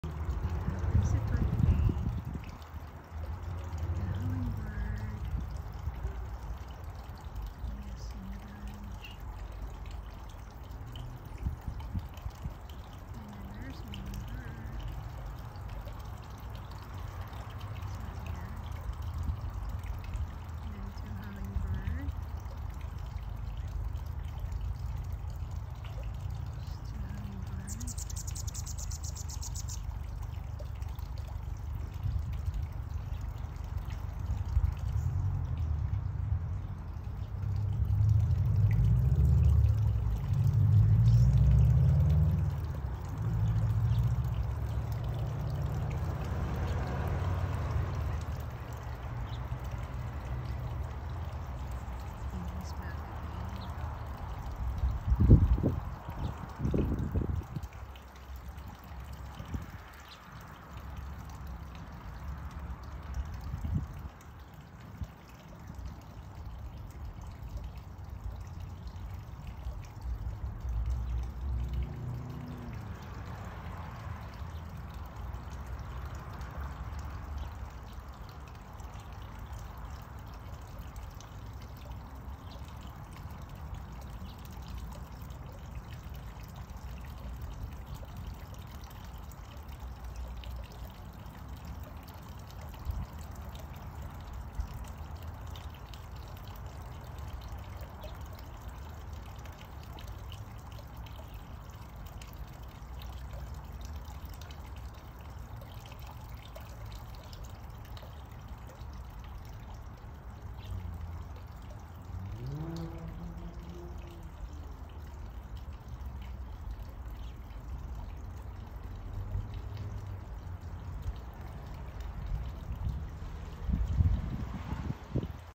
2 Anna’s Hummingbirds and 2 House Finch birds hanging out in my backyard tree.
It was late afternoon just chillin in my back porch and the birds are keeping me company.